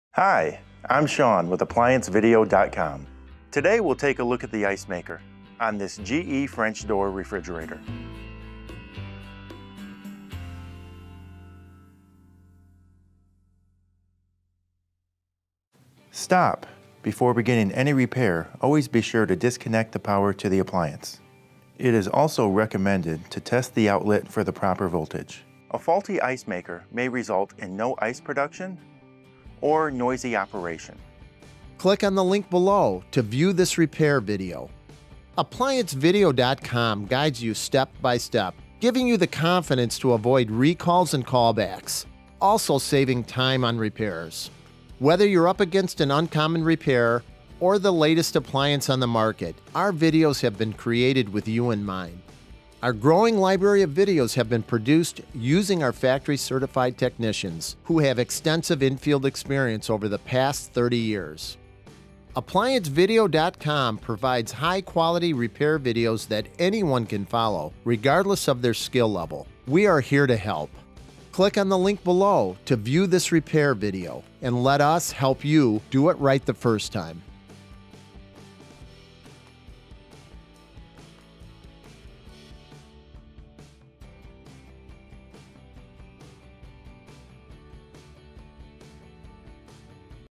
GE Refrigerator - No Ice Production - Ice Maker Repair and Diagnostic
This is a preview of a video that shows you how to Diagnose and Repair a *GFE28HMKES GE Refrigerator WR30X10160 Ice Maker*
*Symptoms may include:* No Ice Production or Noisy operation

This *GE* repair could apply to multiple brands: such as General Electric (GE), Haier, Hotpoint, Cafe, Profile, JC Penney and Monogram